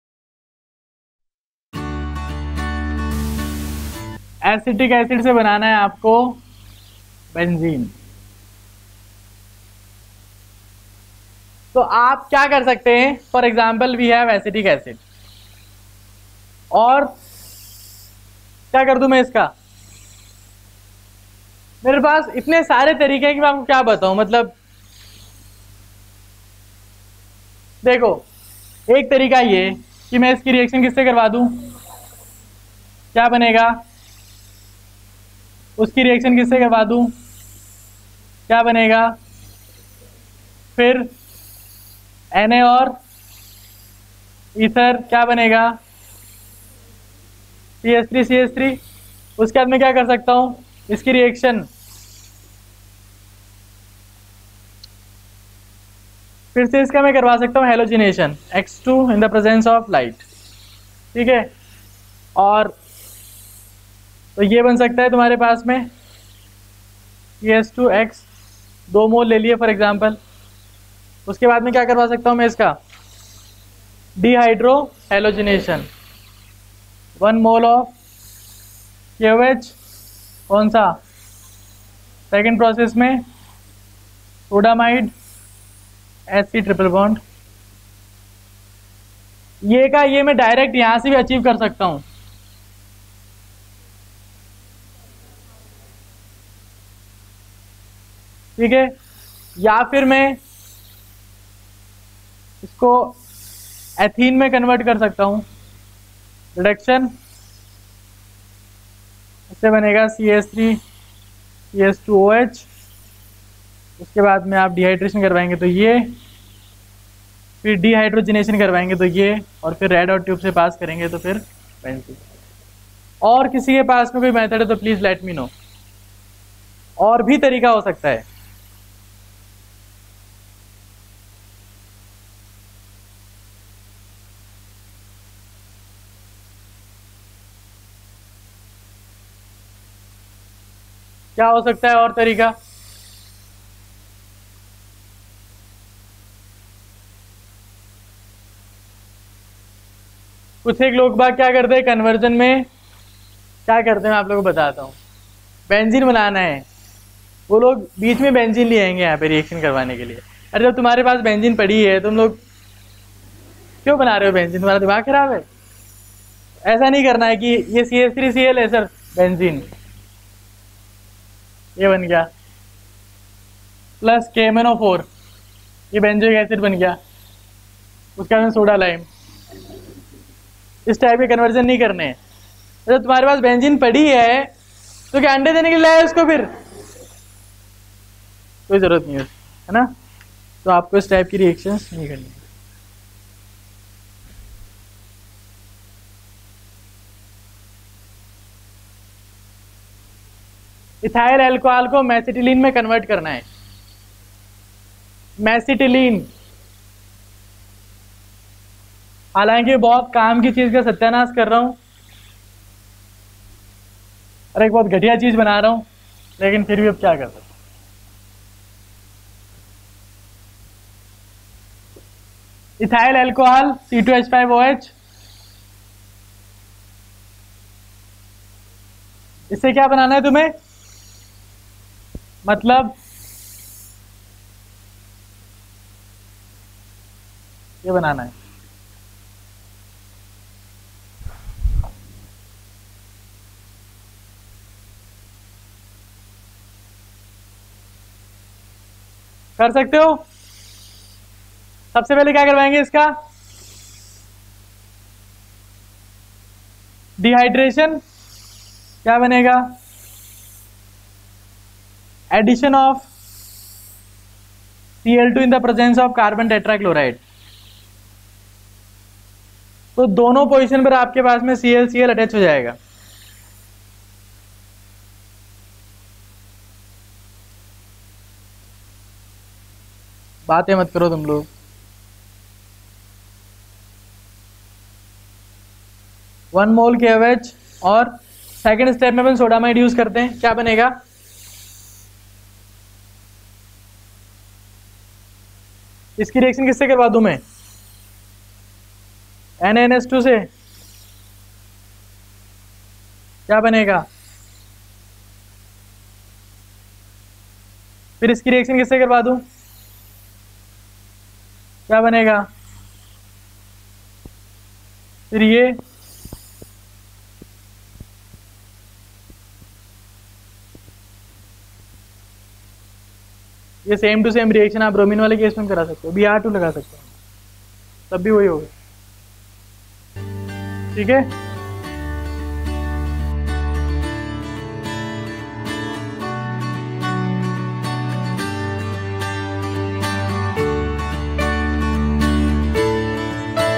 IITJEE | AIIMS : - Lecture 9 Conversion Problems of Benzene
Conceptual Chemistry for IIT JEE | AIIMS

TOPIC : Conversion Problems of Benzene

for more information & updates